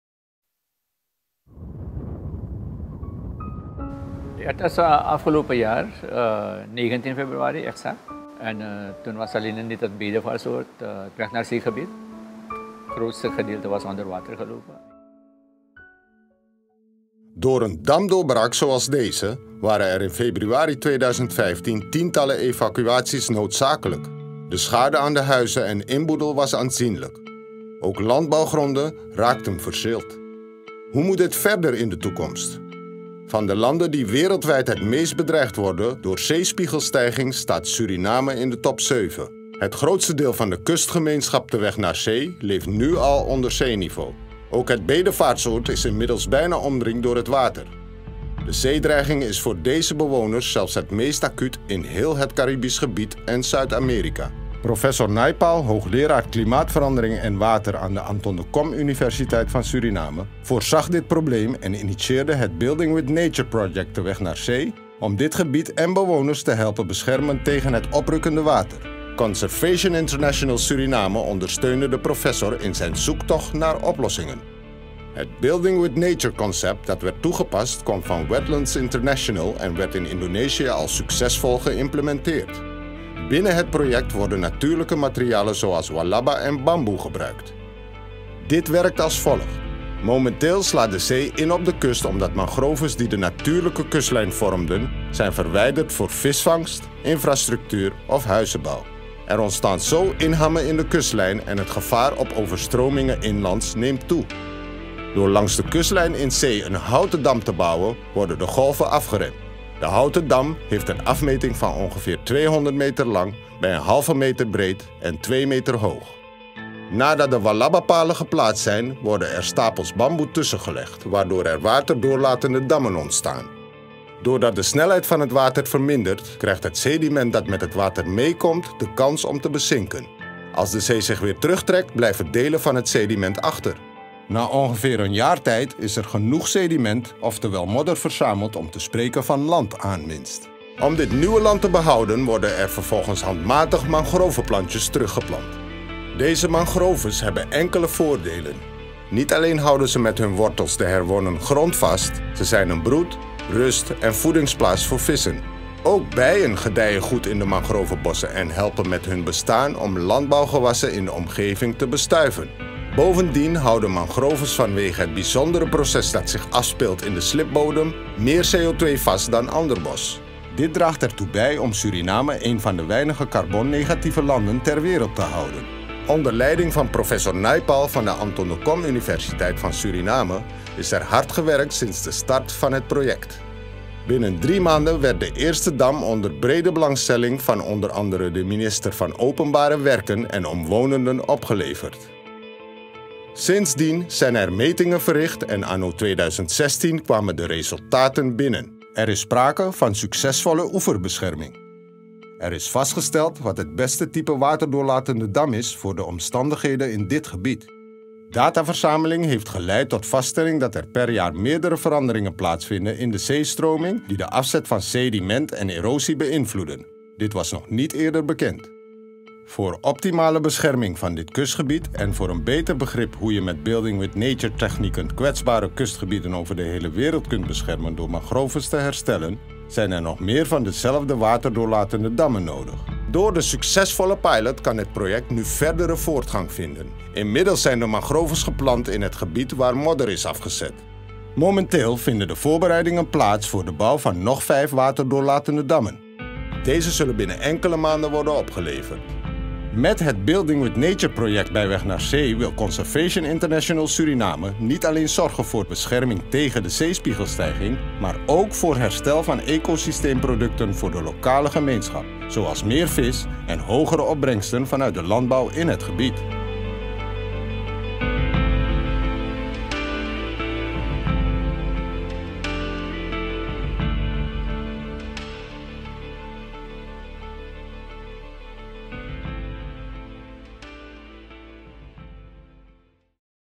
Building with Nature project in Suriname
Conservation International Suriname supported Professor Naipal (Anton de Kom University of Suriname) in his Building with Nature Project at "Weg naar Zee". This is momentarily the most endangered community by sea level rise in the whole of South America en the Caribbean. Wooden, water penetrable, dams are being build to trap sediment and replant mangroves. This project is a successful example of climate change adaptation. (Building with Nature is a technique introduced and successfully implemented in Indonesia by Wetlands International)